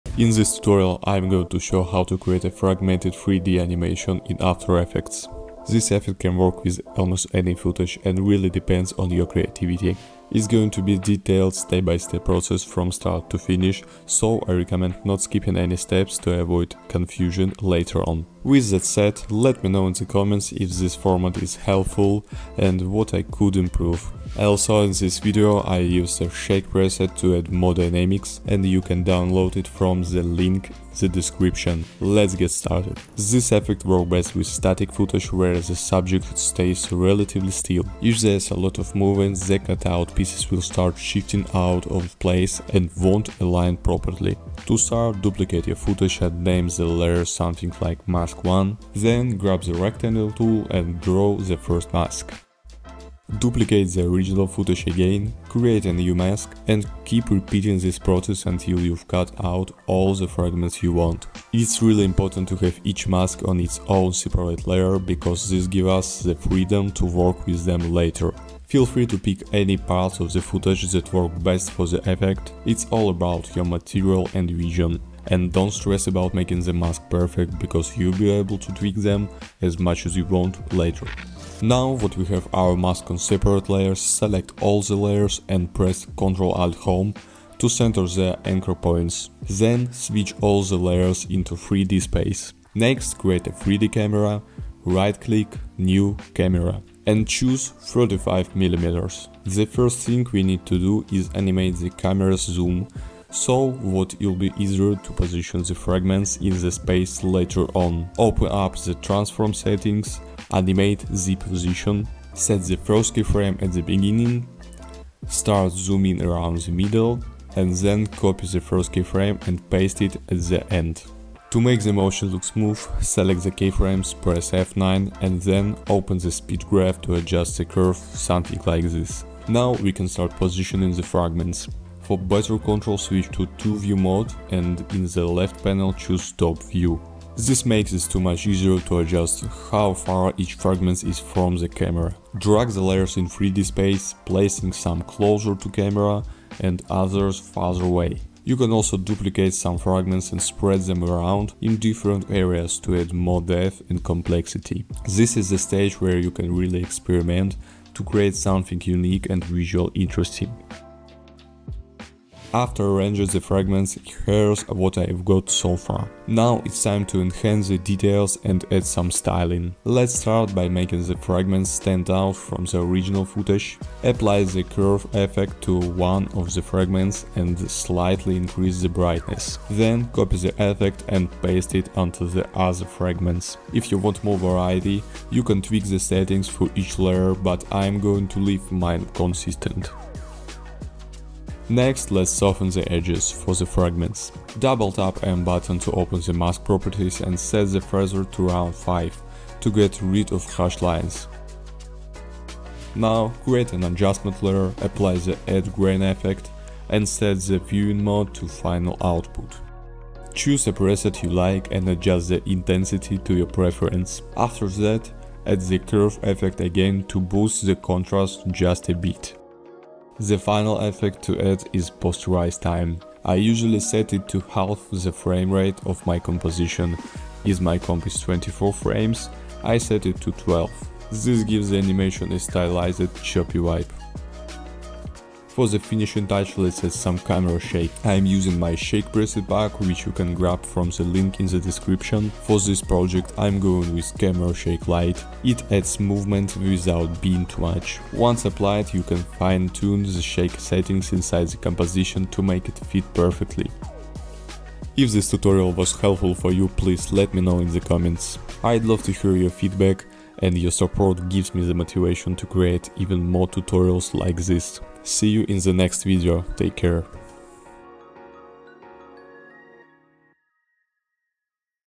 Crazy Fragmented 3D Animation in After Effects (Tutorial)
Timecodes:
0:00 Intro
0:33 Masking
1:28 3D Camera
3:23 Styling

Don't read:
3d animation, video editing, motion design, AI video tools, After Effects tutorial, premiere pro, Alex Arts, AlexArts, Shake effects, motion graphics, video effects